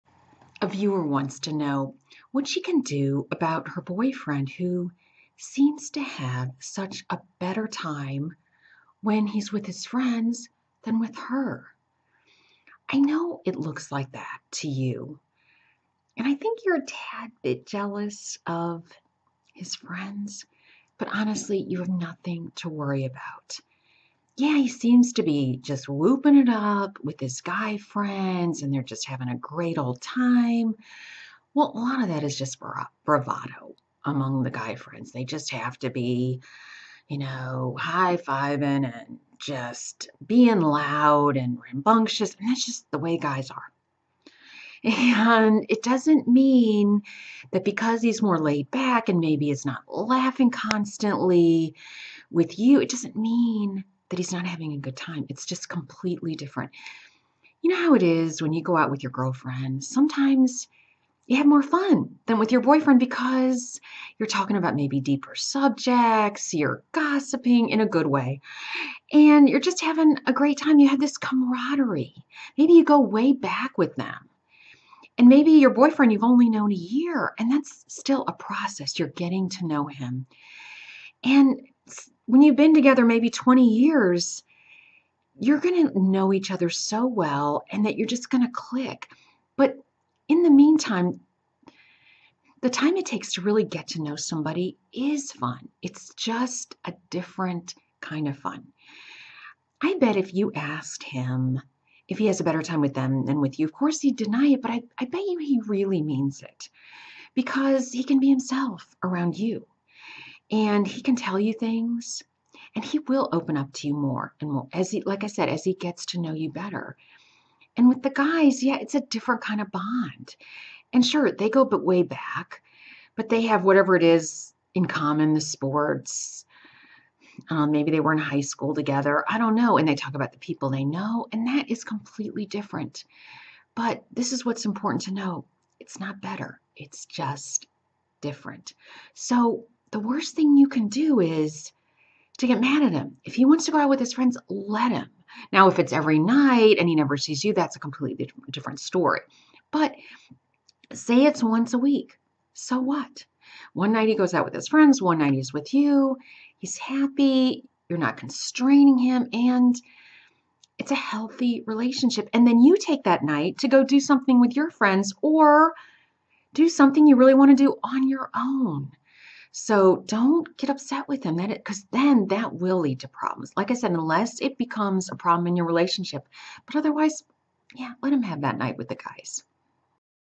What to do when your boyfriend seems to have more fun with his friends than with you
It sounds almost like you are jealous of your boyfriend's friends! And understand, because he has this camaraderie with them that he doesn't have with you. The way you should look at it is, those relationships are completely different. While he may be whooping it up with them, with you, he might be able to open up more, be himself. With them, he can let off steam and create a bond. Whatever you do, I wouldn't complain about his time with the guys (unless it is seriously cutting into time with you.) Give him the one night out a week, and you do the same. Bonding with friends is important.